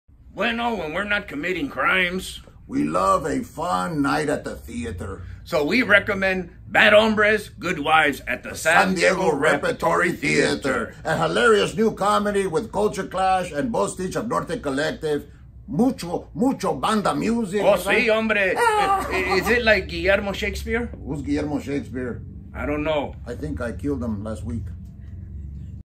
The bad hombres of our world premiere comedy by Herbert Siguenza, BAD HOMBRES/GOOD WIVES, from Oct 3
Our Playwright-in-Residence and Culture Clash member, Herbert Siguenza, and Ric Salinas, also of Culture Clash, may or may not have bribed us to take this video.